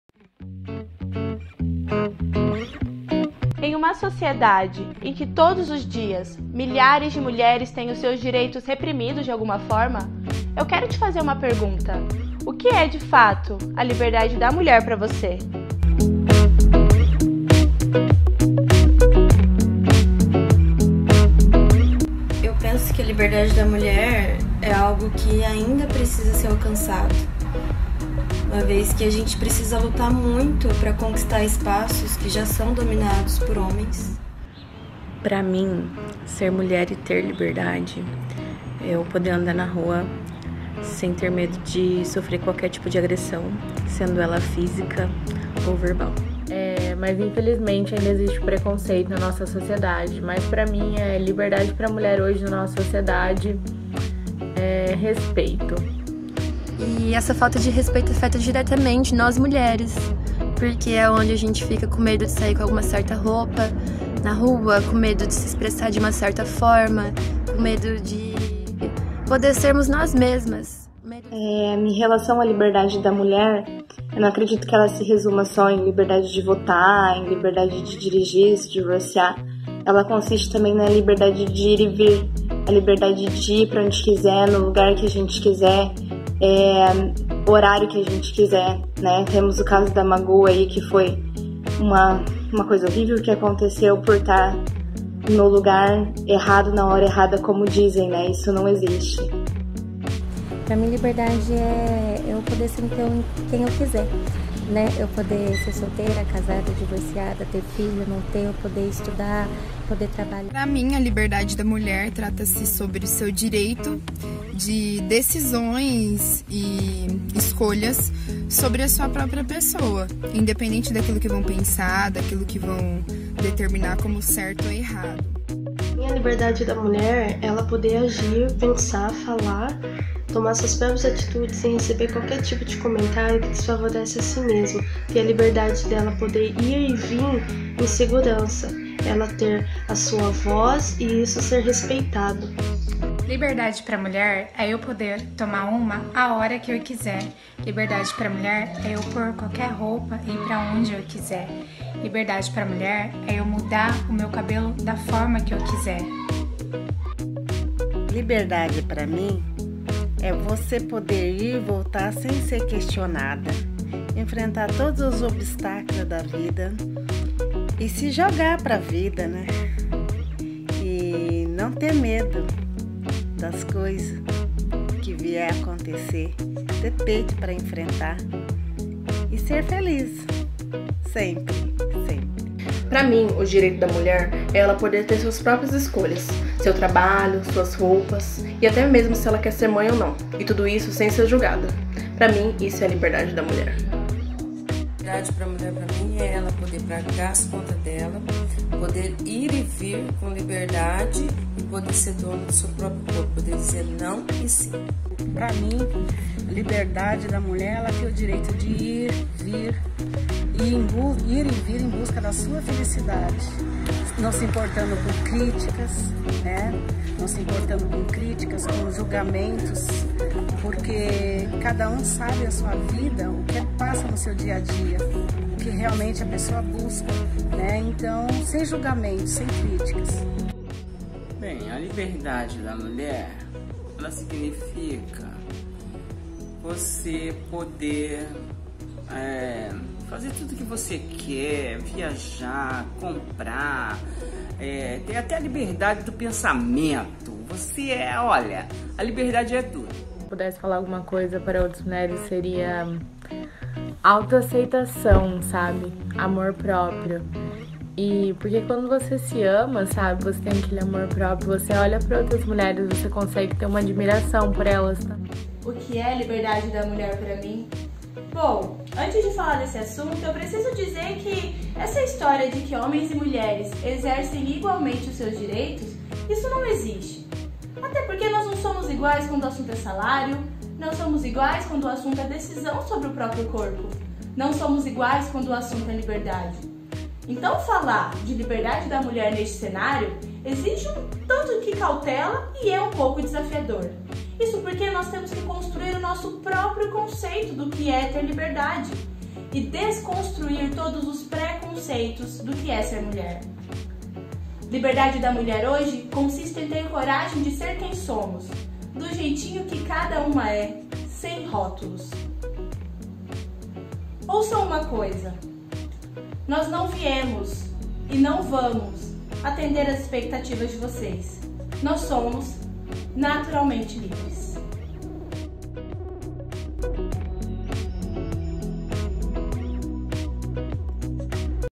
É Meu Direito de Mulher! O que é a Liberdade da Mulher pra você?
Mulheres de todos os tipos e de todas as idades falam de um assunto comum: LIBERDADE FEMININA. Venha ver o posicionamento de cada uma delas e se una a este grupo. Deixe a sua resposta aqui nos comentários: O que é a Liberdade da Mulher pra você?

Venha saber mais sobre os seus direitos no nosso instagram @e.meu.direito.

É Meu Direito Oficial!
É Meu Direito Saber!